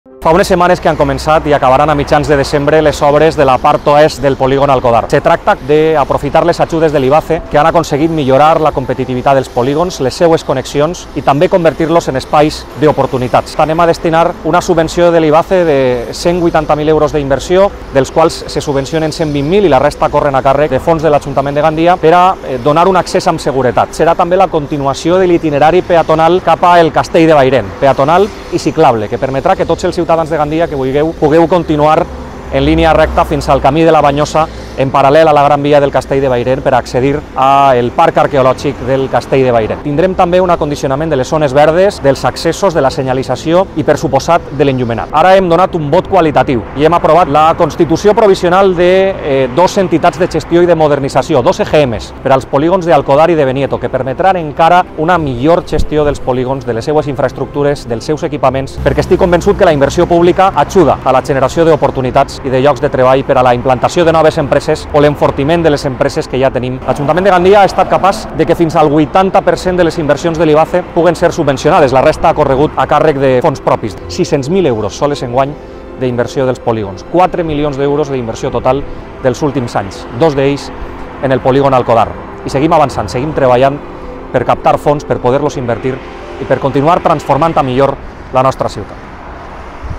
Modernització i millora al polígon Alcodar amb una inversió de 180.000 euros
L’alcalde de Gandia, José Manuel Prieto, acompanyat de la coordinadora general d’Urbanisme, Maite Alonso, tècnics municipals i responsables de l’empresa encarregada de les obres, van visitar els treballs de millora, modernització i dotació d’infraestructures i serveis que s’està realitzant a la part oest del polígon Alcodar. Una actuació que s’ha iniciat recentment i està previst que finalitze a mitjans de desembre. La intervenció té un pressupost de 180.000 euros, dels quals 127.000 procedixen d’una subvenció de l’Ivace mentre que la resta, 53.000 euros, procedixen de l’Ajuntament de Gandia.

L’objectiu dels treballs és millorar la mobilitat i la sostenibilitat d’este polígon industrial amb l’execució de la continuació del carril bici. Una vegada superada la subestació elèctrica, s’executarà el nou tram de carril bici en paral·lel a la Gran Via del Castell de Bairén (antiga N-332) fins al camí de la Banyosa, la qual cosa millorarà l’accessibilitat al Castell.

Paral·lelament, es reordenen els accessos a l’estació de servei existent per millorar la seguretat en este punt, alhora que s’incrementen les zones verdes amb la plantació de 15 arbres i plantes arbustives, es millora la xarxa de clavegueram i la senyalització, i es canalitza l’enllumenat.